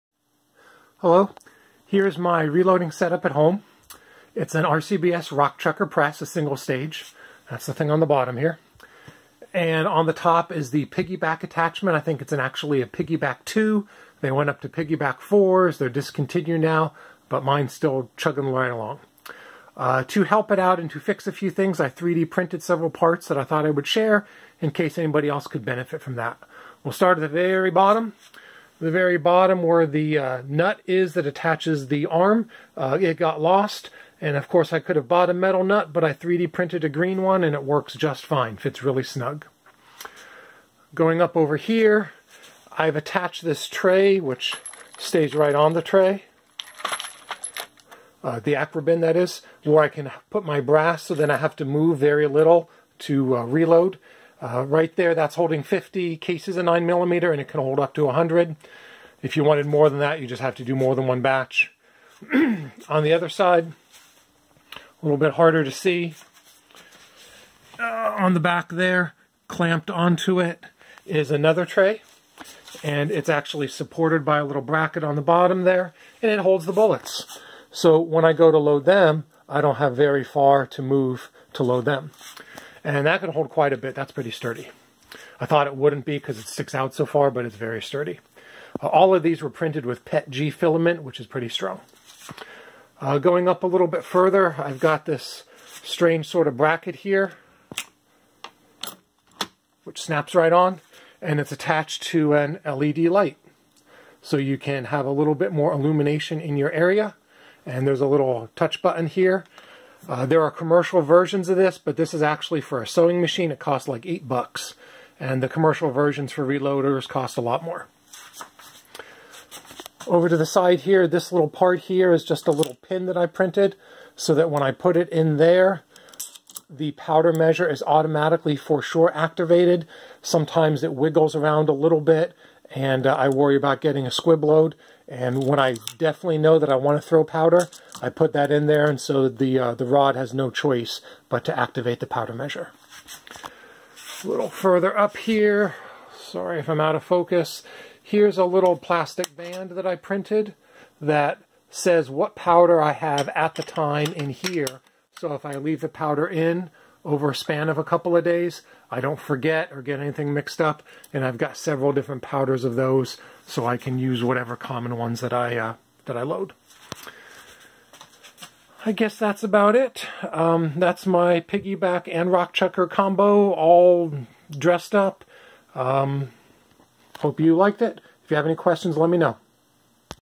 RCBS rockchucker and piggyback II 3D printed accessories
More progressive reloaders have a "red" or "blue" press.   I have a Frankenstein combination of a RCBS single stage rock chucker and a piggyback II attachment that turns it into a progessive press.  The piggyback is now discontinued, but I continue to use mine.   To improve functioning, I added a few parts that I 3D printed with green PETG filament on my Ender 3 printer.   Here are the items described in the video:

1.  A nut to replace the missing one on the handle
2.  A tray that clamps onto the akrobin to hold up to 100 pieces of 9mm brass for ergonomic use.
3.  A tray that clamps onto the back of the rockchucker to hold lots of bullets.
4.  A small pin to ensure powder dispensing when needed.  This ensures it will not go into the position to skip throwing powder, thus avoiding squib loads.
5.  A bracket that clamps onto the top of the piggyback where I hot glued a strip of LED lights.  This was a sewing machine light strip that was only about $8 on amazon.
6.  A ring on the top of the powder dispense that designated what powder I'm using in case there is confusion.

Not mentioned in the video:
7.  The mostly clear powder drop tube cracked and broke after years of use.  After several ignored emails to RCBS I just printed my own.  It isn't completely clear, but it works well enough.  That part with with clear PLA filament.

All of the parts were originally designed, or at least heavily modified, on Tinkercad, than sliced with Cura.

If you have a simliar setup to this, and have any questions or use these ideas, please let me know.